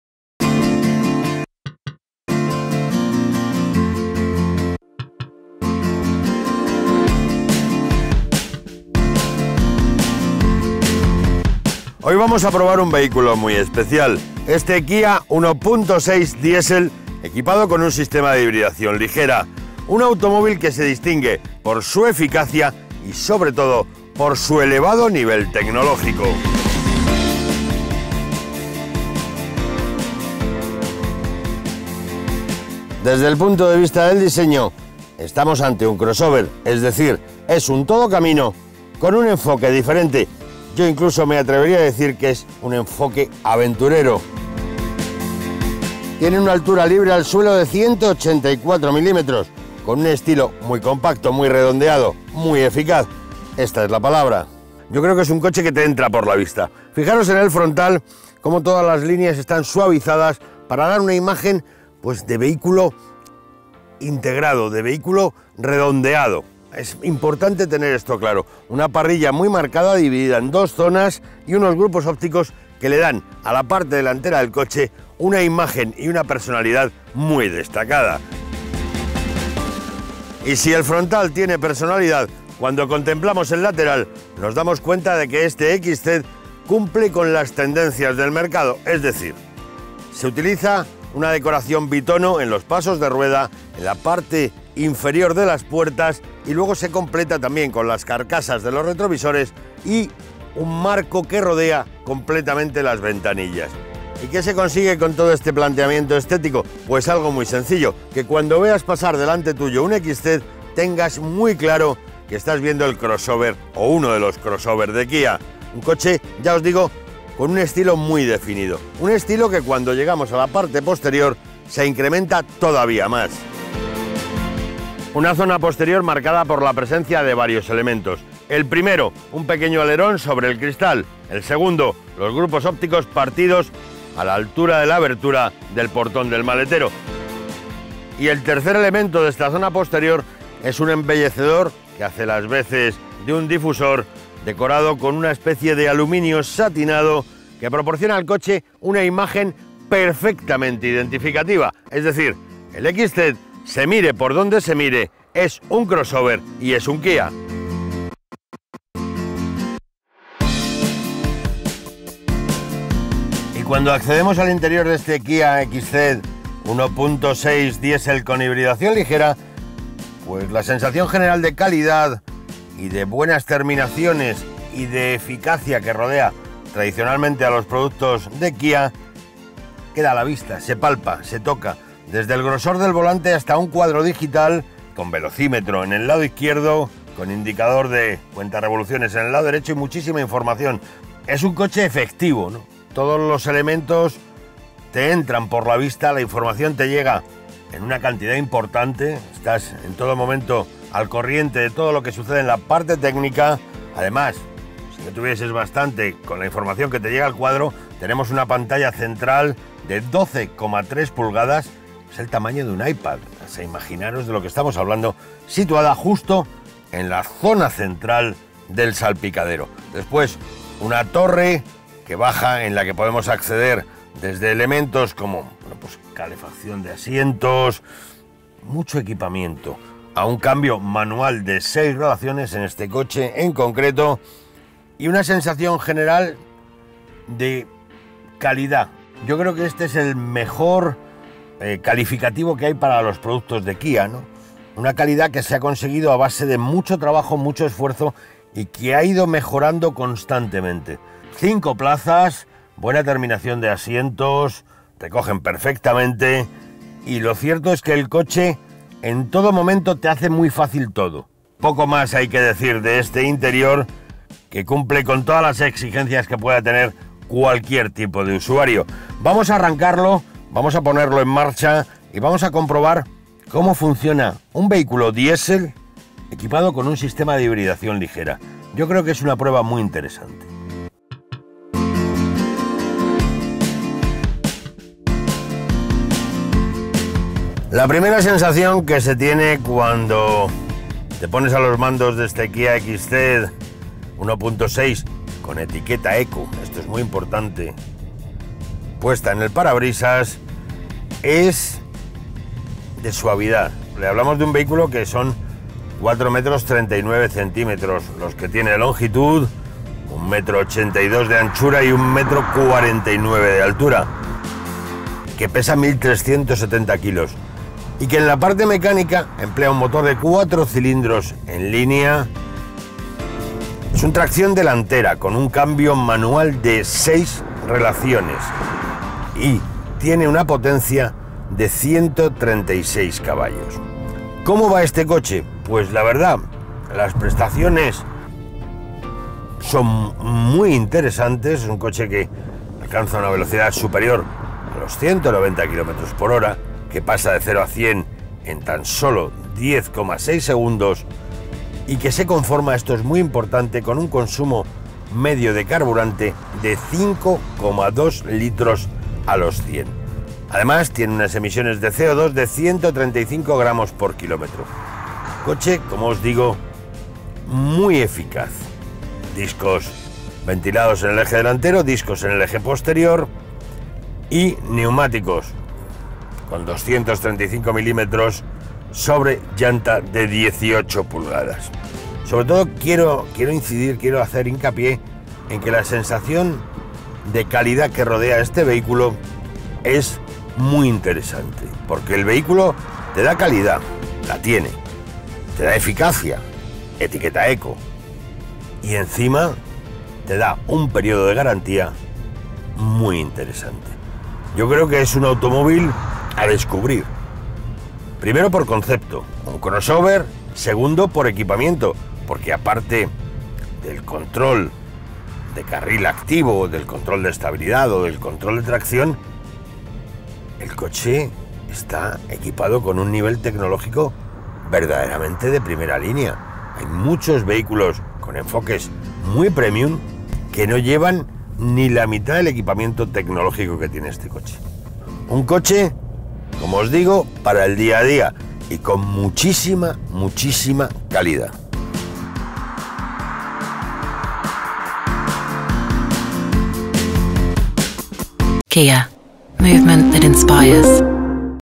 Kia | Así fue la prueba del Kia XCeed 1.6 MHEV con Gonzalo Serrano
¿Qué se siente al conducir el KiaXCeed 1.6 MHEV? Ahora es el momento de que lo sepas gracias al análisis que hace Gonzalo Serrano, periodista de Más Que Coches. ¡No te lo pierdas!